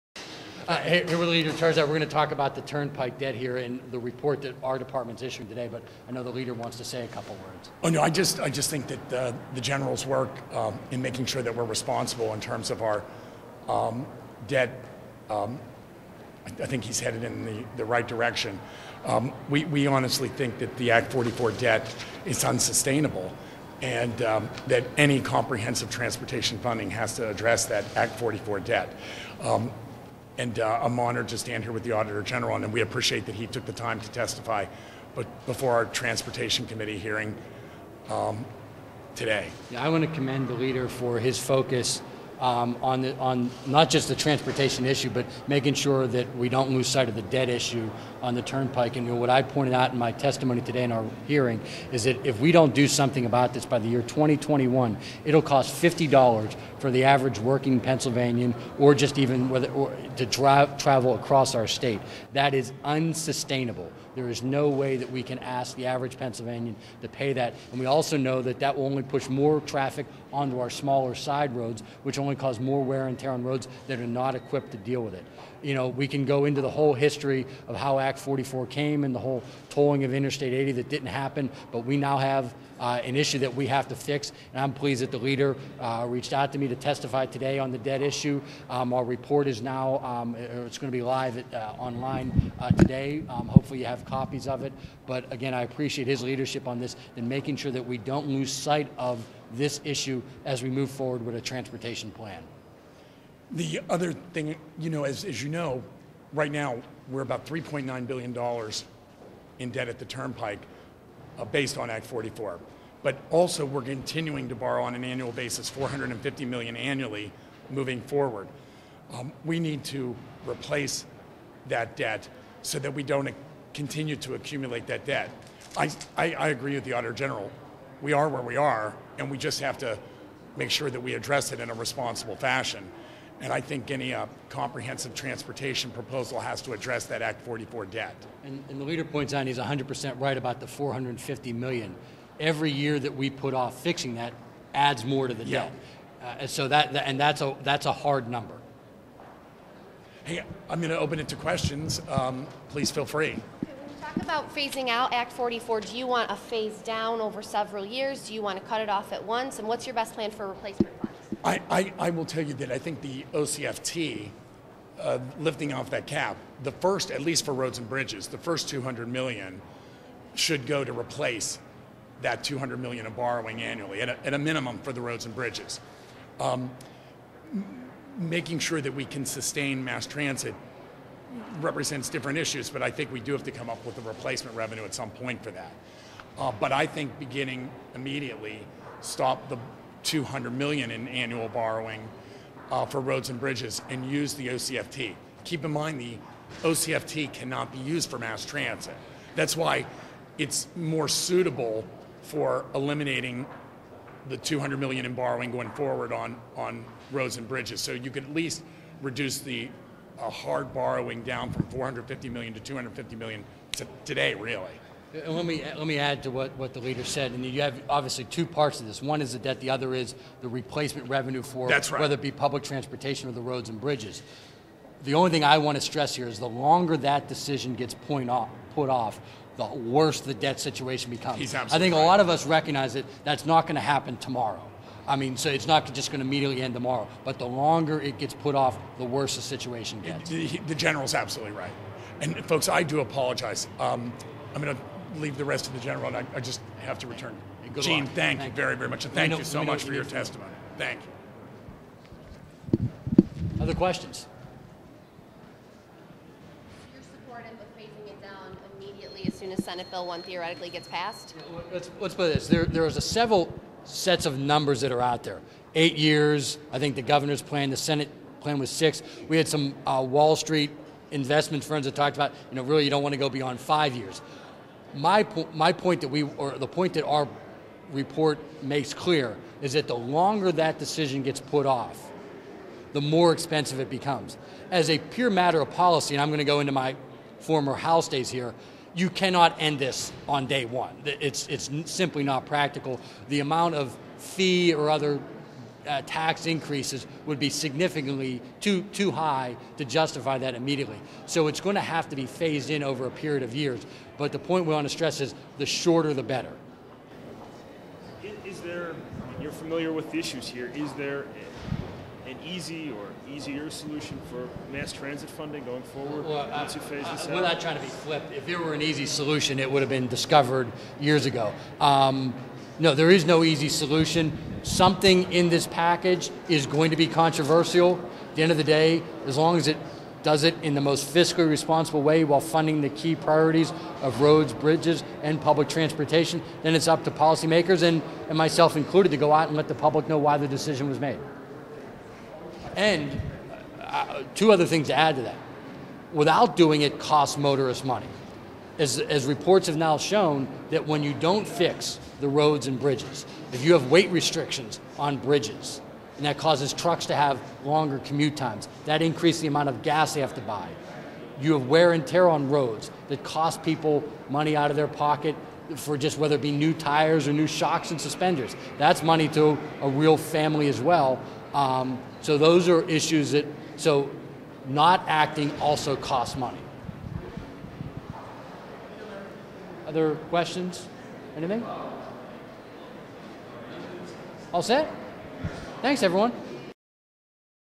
Turnpike Debt Must Be Resolved
Rep. Mike Turzai is joined by Auditor General Eugene DePasquale to discuss the Turnpike's debt situation.